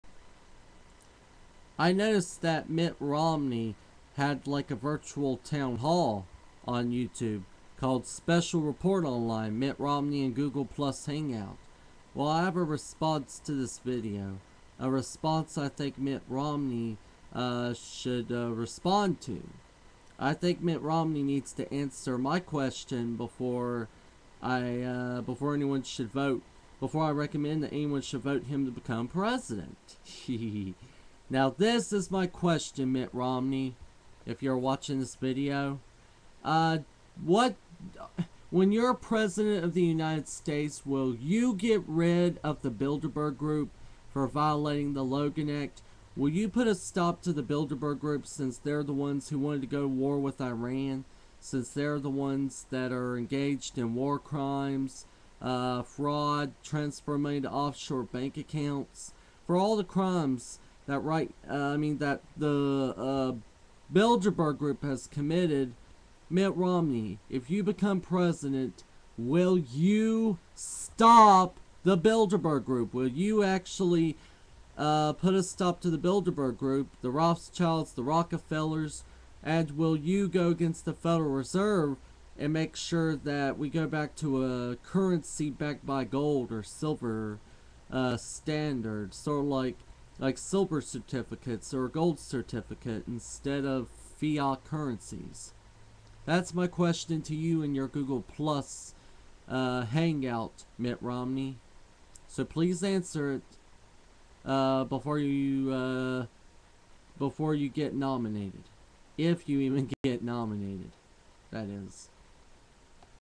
Re: Special Report Online: Mitt Romney in Google+ Hangout
USWGO Founder Brian D. Hill has decided to ask Mitt Romney and his Google+ virtual town hall by Fox News if he will get rid of the Bilderberg Group and stop the federal reserve

USWGO's webcam video November 18, 2011 08:58 AM